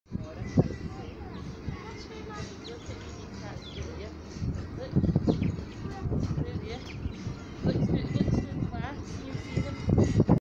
Meerkats
These little guys have a great life at the 5 sisters zoo in Scotland where they share an enclosure with a couple of crane birds and a giant tortoise!

Not your typical zoo either, but a rescue centre for abused animals, so check it out and donate.